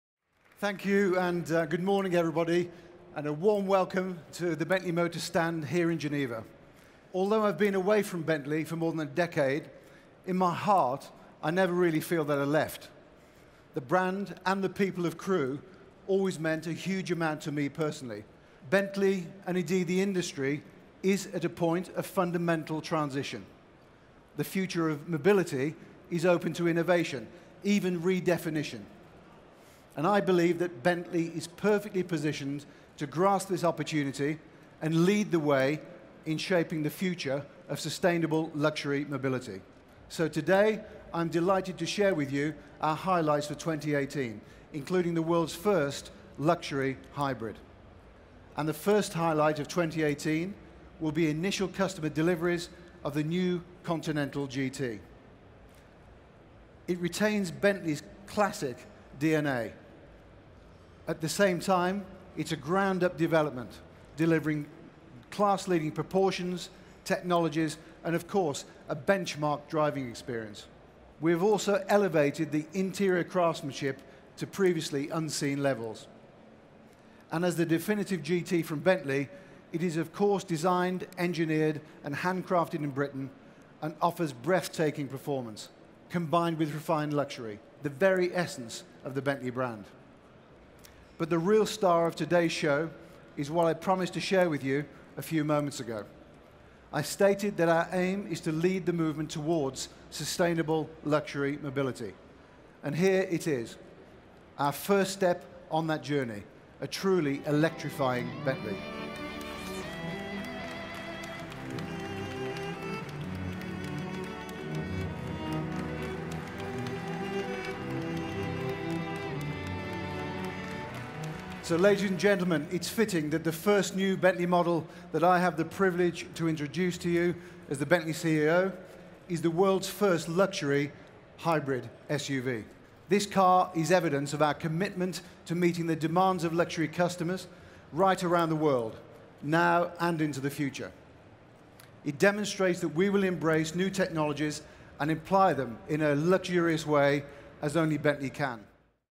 Bentley Press conference at 2018 Geneva Motor Show
Bentley’s new Chairman and CEO, Adrian Hallmark, used his first public appearance with the luxury British brand to unveil an exciting new model in Geneva. Alongside this global debut was the Bentayga V8, which also made its first motor show appearance.  Joining the Bentayga was the all-new Continental GT, on show in Geneva for the first time.